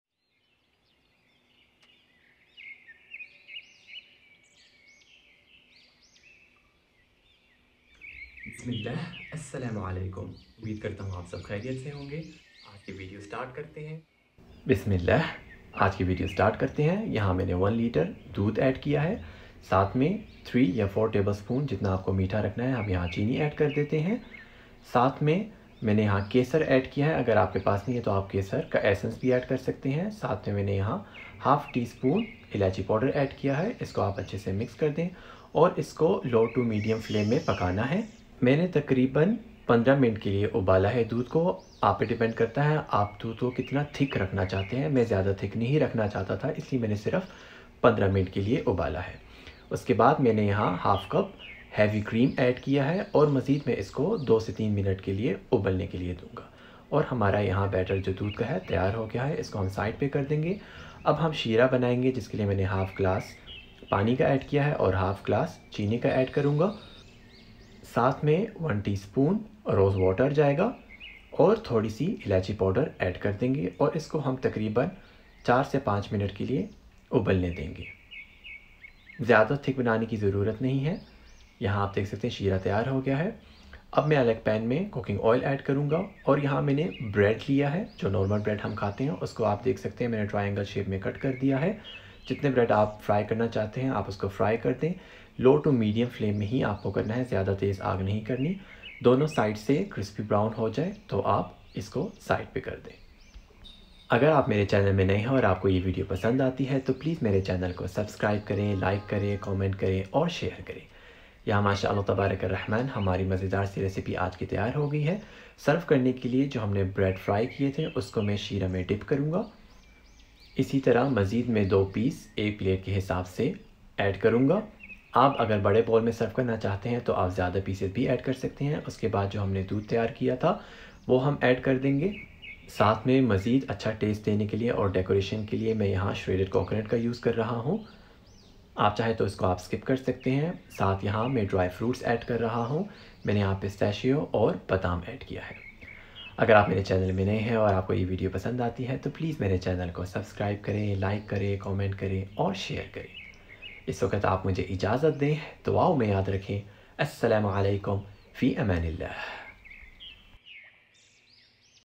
How To Make Shahi Tukrey 🥰شاہی ٹکڑے🥰 Milk Bread Dessert
I am Zam , Pakistani Lebanese Guy 😉

In this video i ll share how to make Shahi Tukrey

Will enjoy making super delicious food and amazing theme cakes and desserts